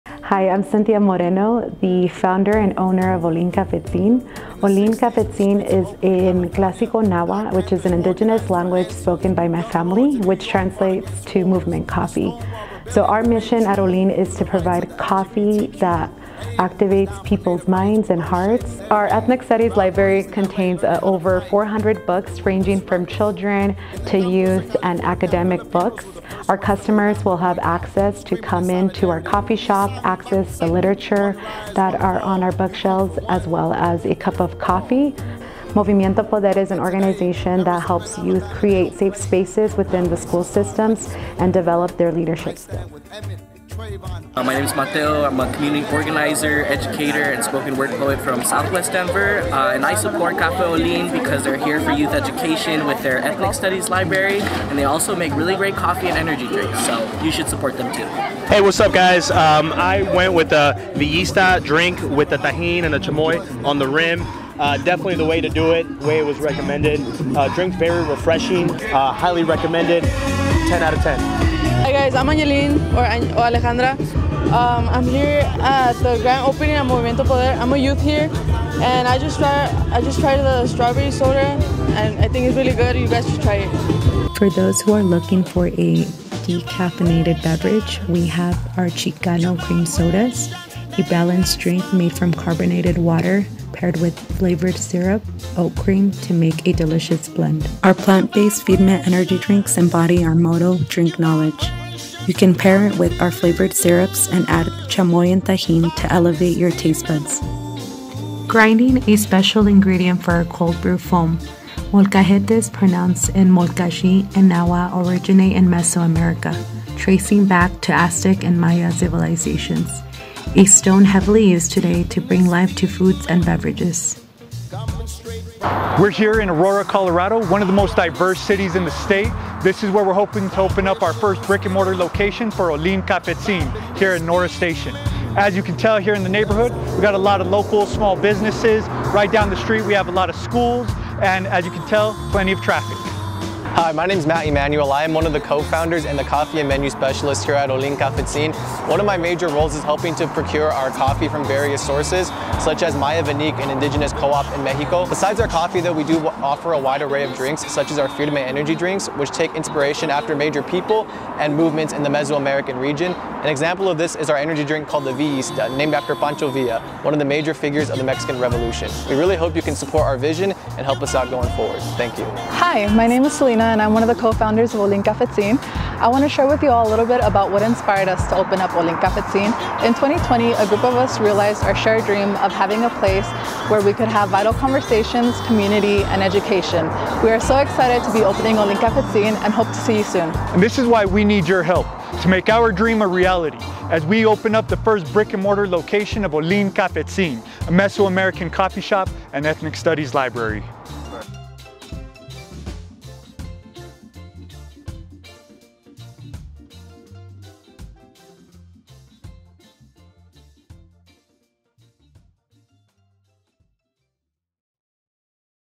Invest in Ollin Cafetzin!
Ollin Cafetzin is opening their first brick-and-mortar location!